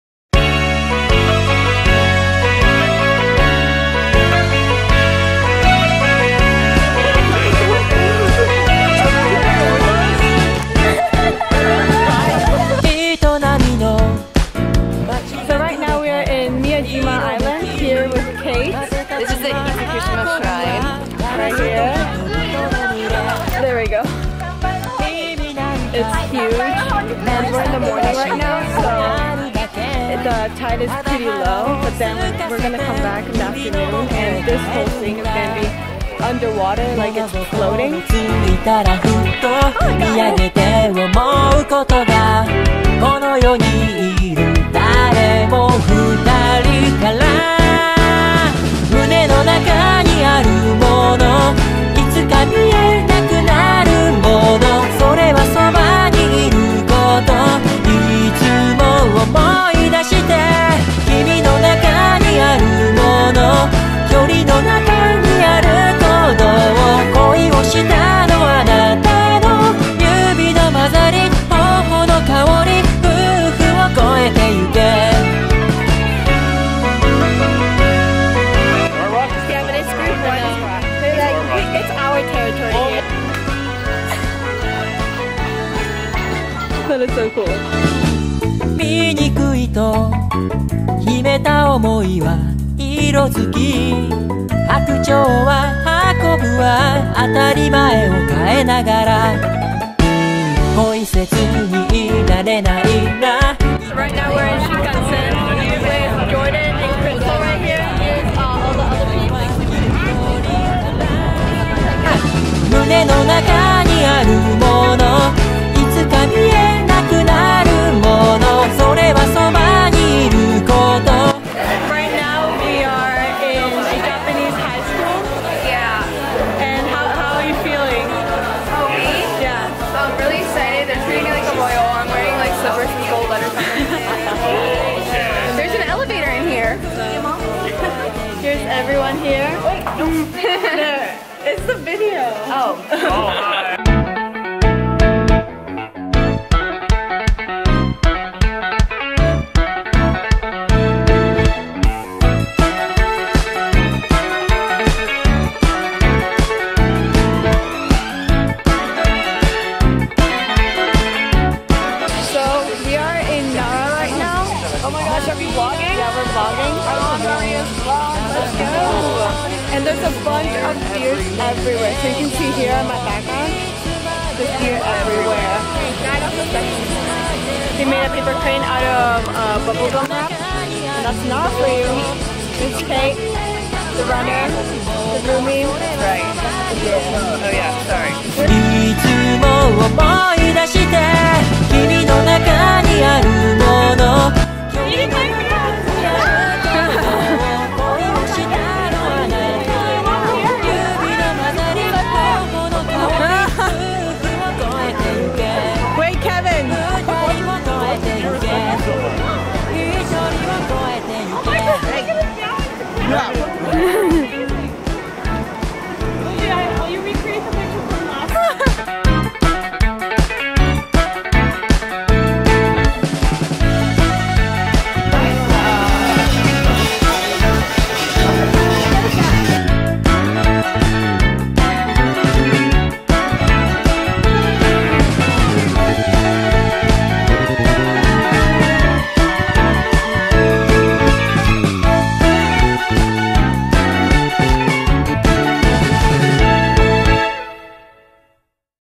Travelling through Japan!
We went to Itsukushima, Osaka, Hiroshima, Nara, & Kyoto!